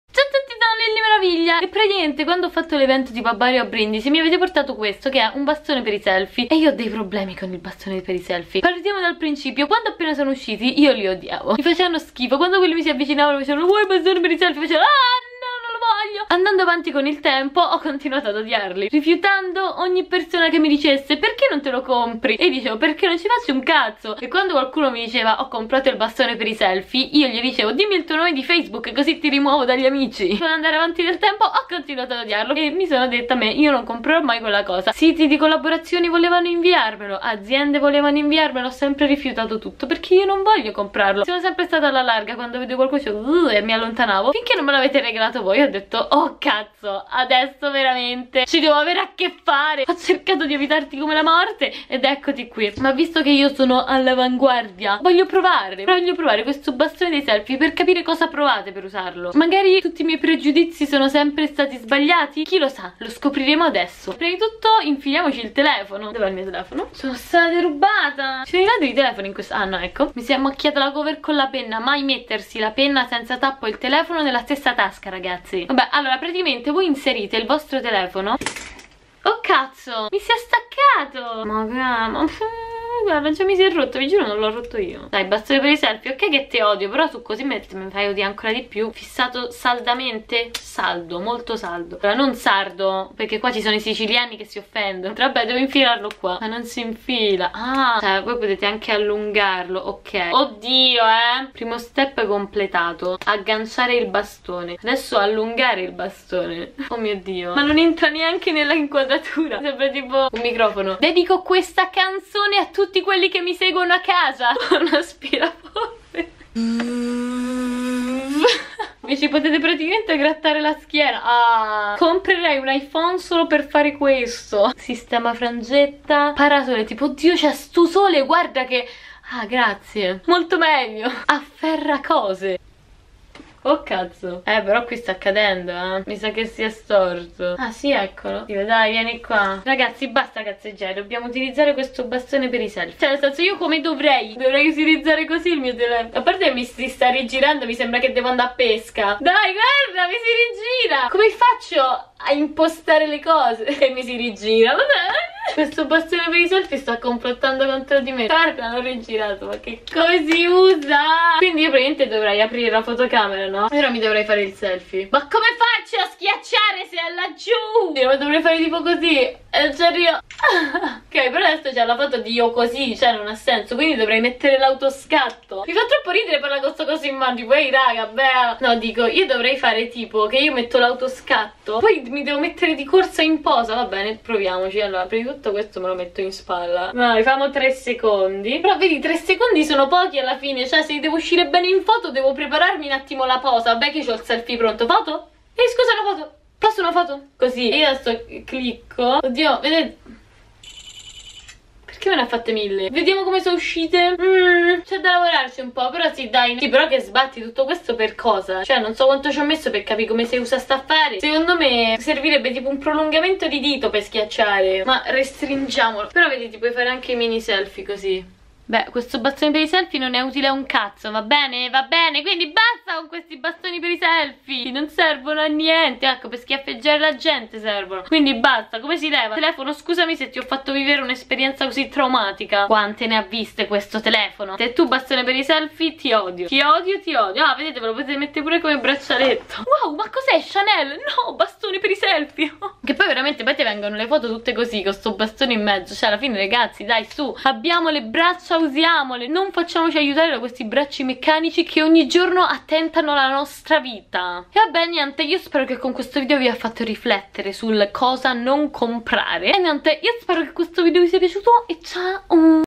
NON SO USARE IL BASTONE DEI SELFIE
LEGGI QUI DANNAZIONE
Non sono pronta per questi nuovi aggeggi
Dove potete trovarmi
Snapchat liviacaramella